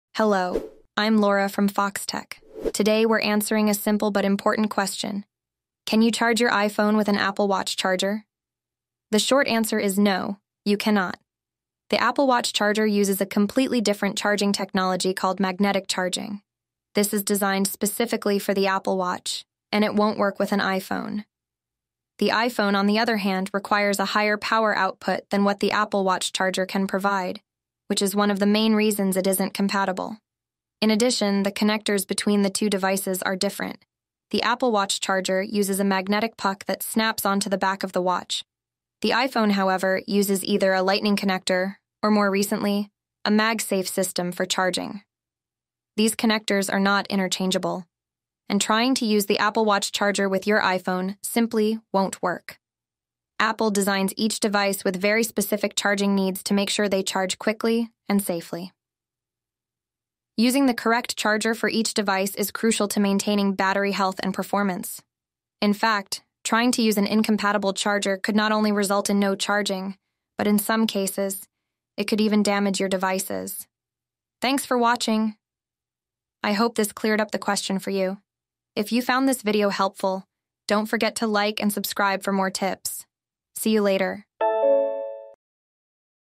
Can You Charge the iPhone from Apple Watch Charger (explained)
In this video, we address whether it's possible to charge an iPhone using an Apple Watch charger. We'll explain how the charging technology works for each device and clarify if this is a feasible option. If you're wondering about the compatibility between your iPhone and Apple Watch charger, this video will provide the answers you need. Ideal for anyone looking to better understand their Apple devices.